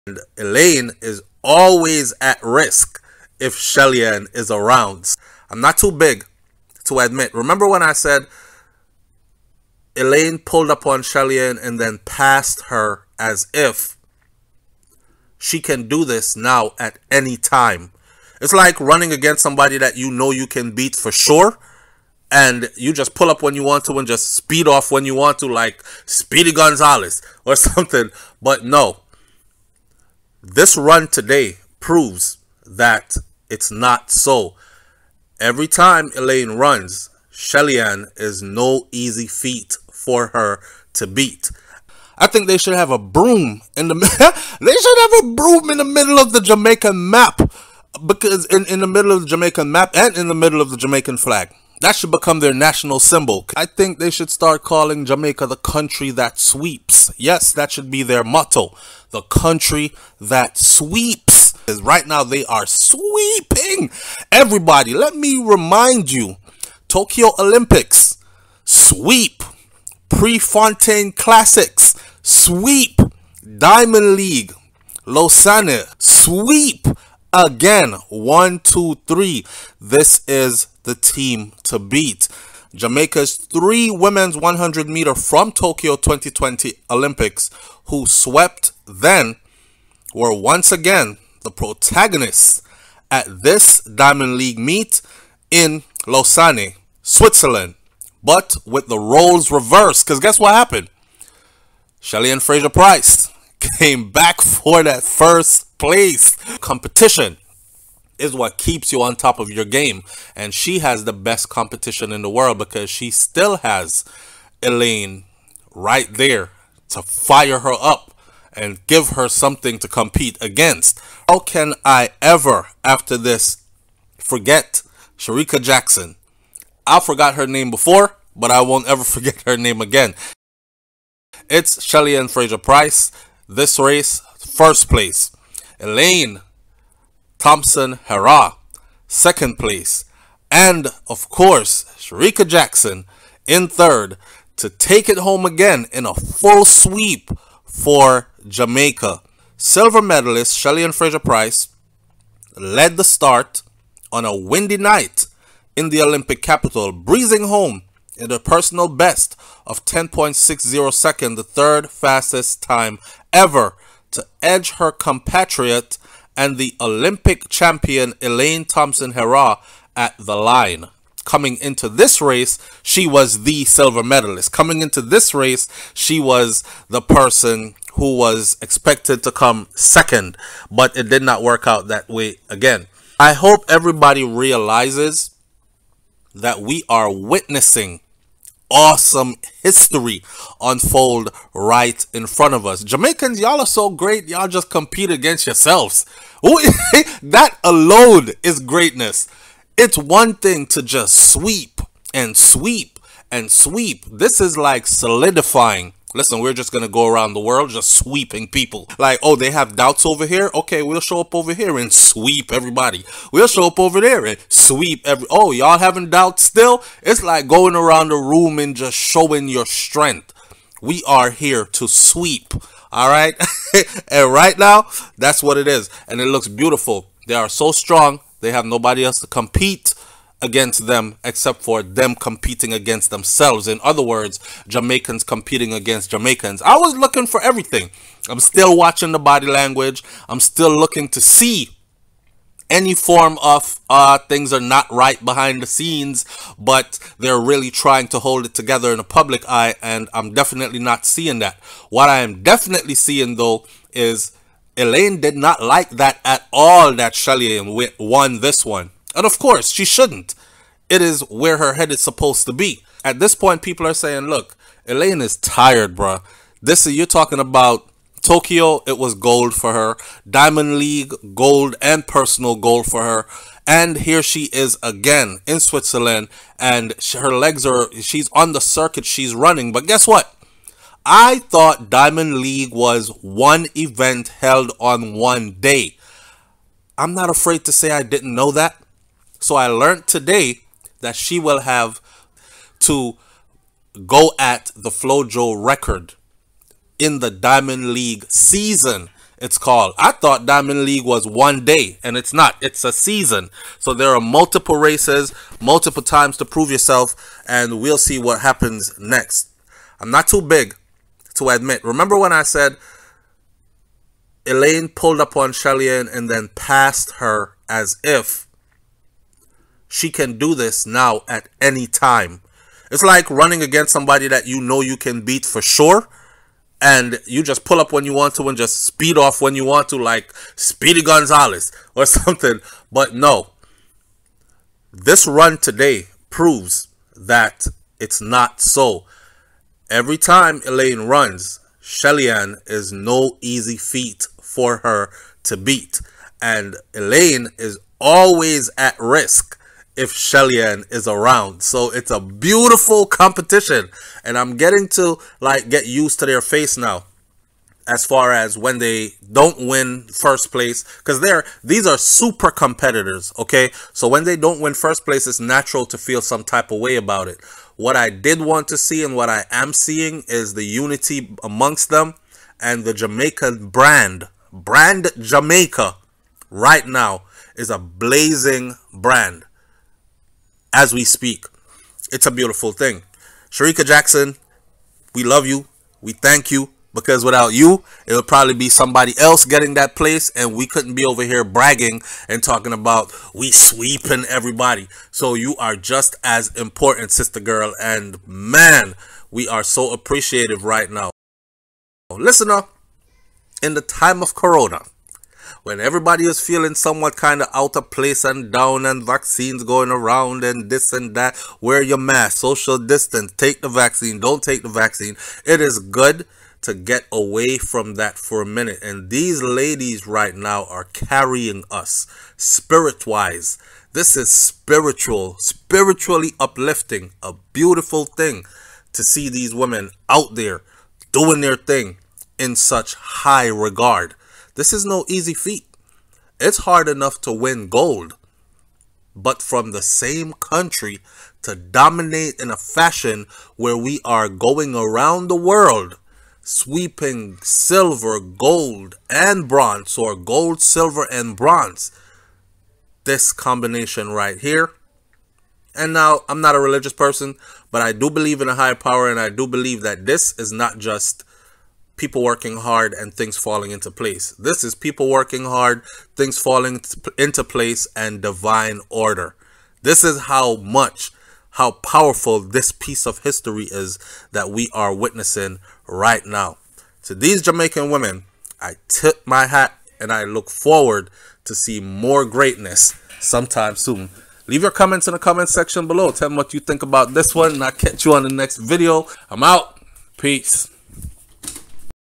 Jamaican Women Dominate Track and Field Sprints 2021 FLASHBACK
Elaine Thompson Herah, Shellyann Fraser Price, and Sherika Jackson are the 3 women to beat in women's track and field sprints as the three Jamaican women have dominated every professional meet attended to include the Tokyo Olympics, the Prefontaine Classics and The Diamond League circuit, for 2021 and in time to come the Jamaican women will be a force to recon with. This video was partially taken from SOUFLOTV and is a reupload so we could relive a glorious and historic moment once again.